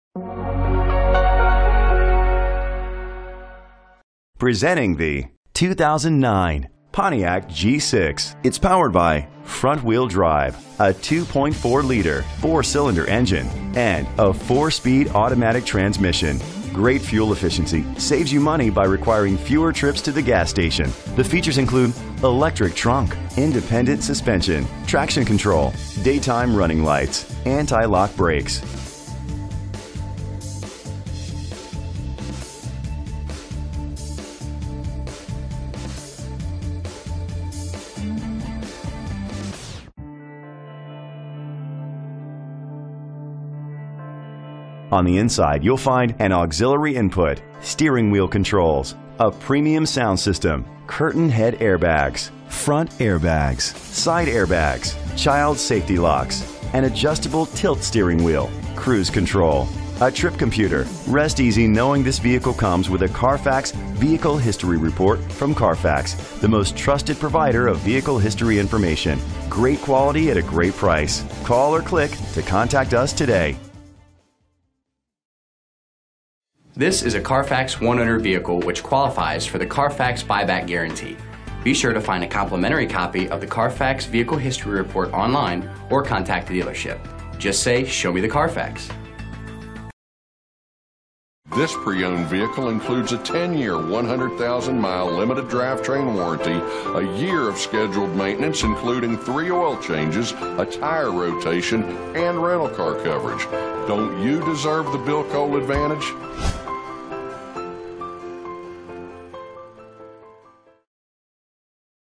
2009 Pontiac G6 Y642B - Princeton WV
Year: 2009
Make: Pontiac
Model: G6
Trim: 4DR SDN W/1SA *LTD AVAIL*
Engine: 2.4 liter 4 cylinder FWD
Transmission: 4-Speed Automatic
Color: Silver
Mileage: 86695
Address:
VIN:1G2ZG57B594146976